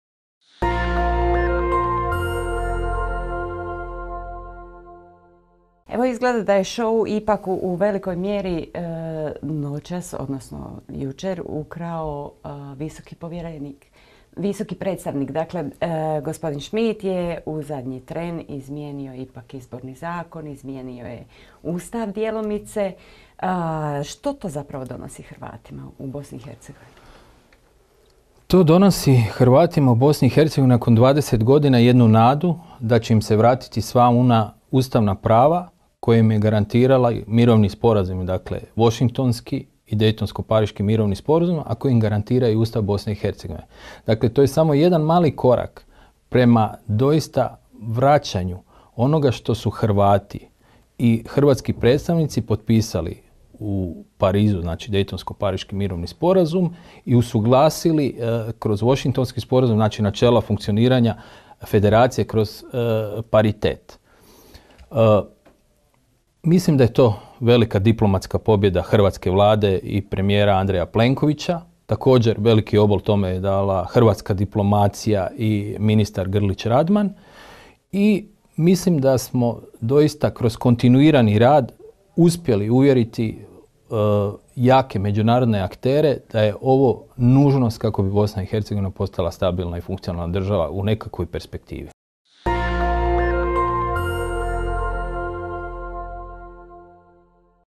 ANALIZA IZBORA: 'Velika pobjeda Plenkovića, uvjerili međunarodne aktere što treba za stabilnu BiH!'
Nakon burne izborne noći u Bosni i Hercegovini, u studiju Večernjeg lista analiziramo što će najnoviji rezultati, ali i intervencija visokog predstavnika Christiana Schmidta značiti za budućnost zemlje. Gost u studiju bio je Domagoj Knežević, znanstvenik s Hrvatskog instituta za povijest i savjetnik ministra vanjskih poslova RH.